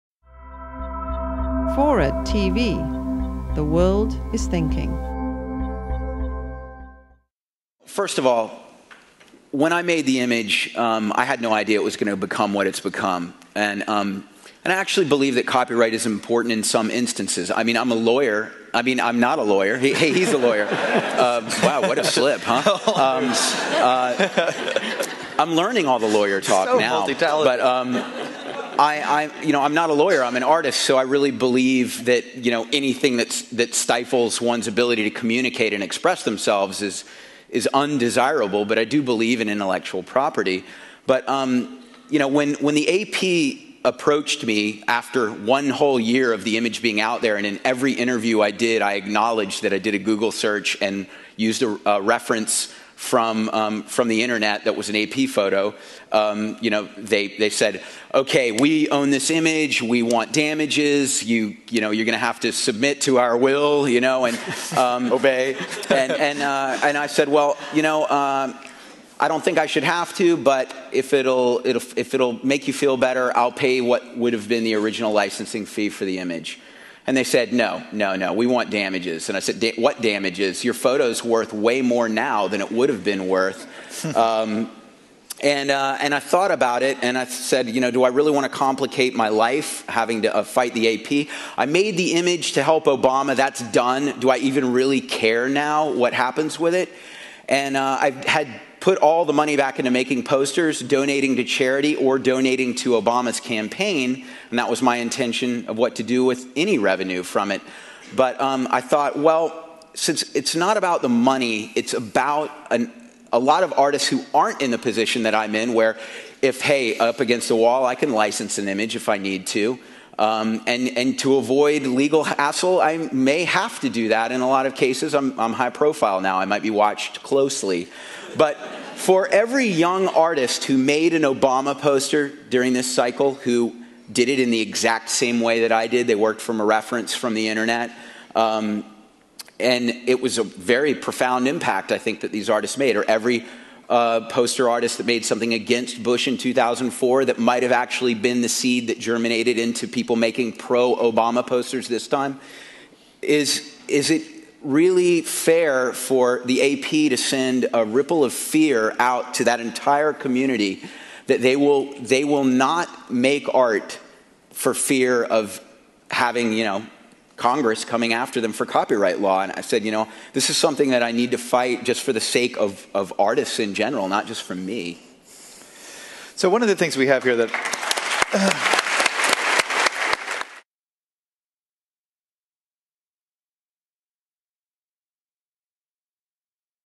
Shepard Fairey on Fighting the AP Over Obama HOPE Image
Shepard Fairey, contemporary artist behind the Barack Obama "Hope" portrait, discusses the reasoning behind his lawsuit against the Associated Press. "I'm not a lawyer, I'm an artist," says Fairey. "I really believe anything that stifles one's ability to communicate and express themselves is undesirable."

What is the future for art and ideas in an age when practically anything can be copied, pasted, downloaded, sampled, and re-imagined?

LIVE from the NYPL and WIRED Magazine kick off the Spring 2009 season with a spirited discussion of the emerging remix culture.

Our guides through this new world--who will take us from Jefferson's Bible to Andre the Giant to Wikipedia--will be Lawrence Lessig, author of Remix, founder of Creative Commons, and one of the leading legal scholars on intellectual property issues in the Internet age; acclaimed street artist Shepard Fairey, whose iconic Obama "HOPE" poster was recently acquired by the National Portrait Gallery; and cultural historian Steven Johnson, whose new book, The Invention of Air, argues that remix culture has deep roots in the Enlightenment and among the American founding fathers.

Shepard Fairey shot to national fame as the graphic artist behind a 2008 iconic poster of Barack Obama, a portrait labeled simply "HOPE" and in a style that could be described as Andy Warhol meets Socialist Realism. Fairey, who graduated from the Rhode Island School of Design in 1992, was already well known among graffiti artists and fans, thanks to one of Fairey's early works of "guerilla" art, an impromptu stencil design based on an ad for Andre the Giant, a professional wrestler.

With permission from the staff of Obama's presidential campaign, Fairey began distributing the "HOPE" image in January of 2008. A year later, with Obama in the White House, Fairey's poster was officially displayed in the National Portrait Gallery in Washington, D.C. Around the same time the Associated Press declared Shepard's poster was based on a 2006 photo taken by the AP's Manny Garcia and they should get credit and compensation. Fairey filed a pre-emptive lawsuit against the AP, arguing he didn't owe them. Fairey has appeared in the documentary films Andre the Giant Has a Posse (by Helen Stickler, first distributed in 1997) and Bomb It! (2007), and his work has been documented in the book Supply and Demand.